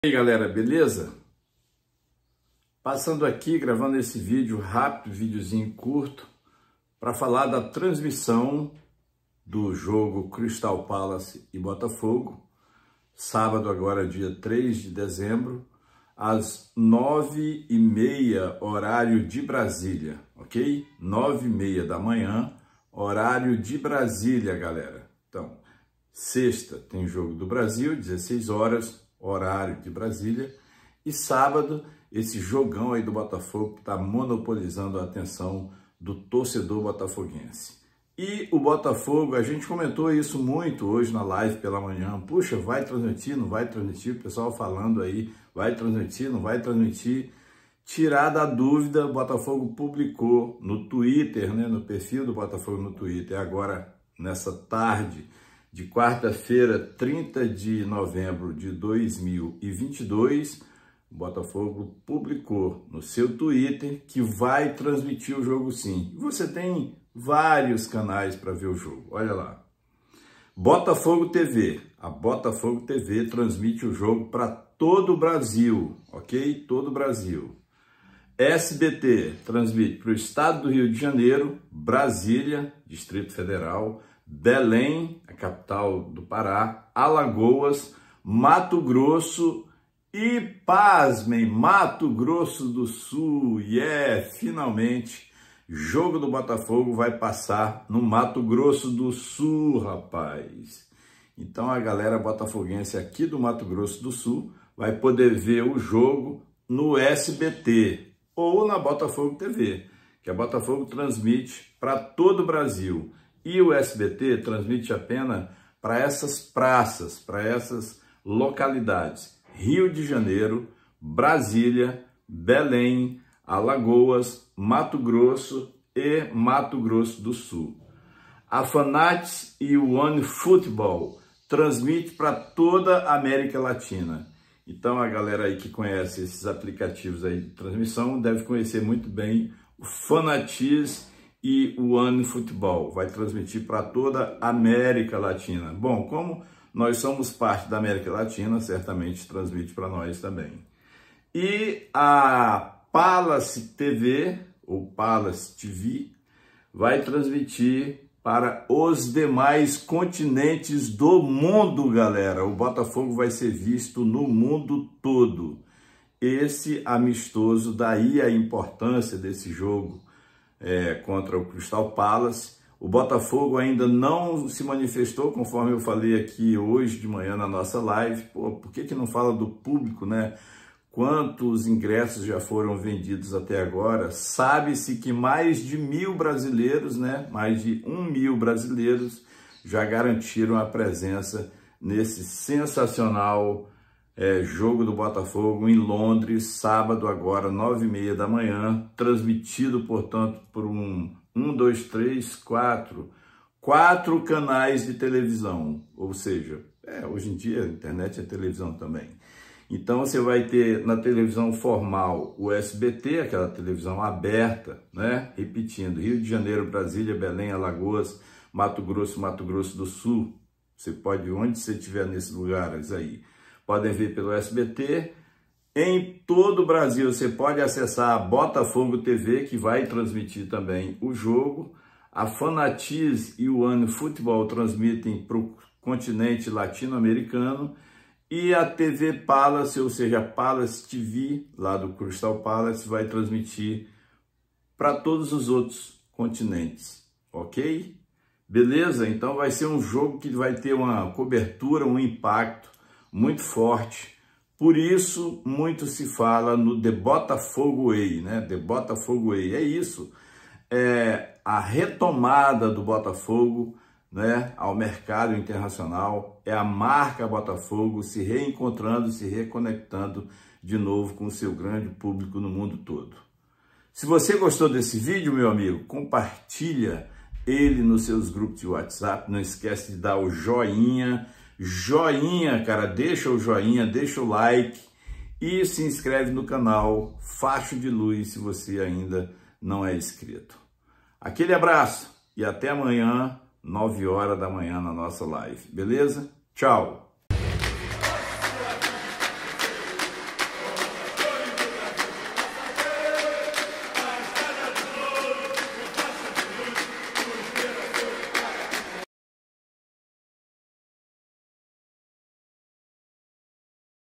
SBT Rio e Botafogo TV vão transmitir direto de Londres o amistoso entre Crystal Palace e Botafogo
SBT e Botafogo TV transmitem o jogo do Botafogo com Crystal Palace. Além deles, os aplicativos Fanatiz, One Futtbol e a Palace TV também transmitirão. O jogo vai poder ser visto no mundo todo. É o Fogão se reconectando com seis mais de seis milhões de torcedores em todo o mundo!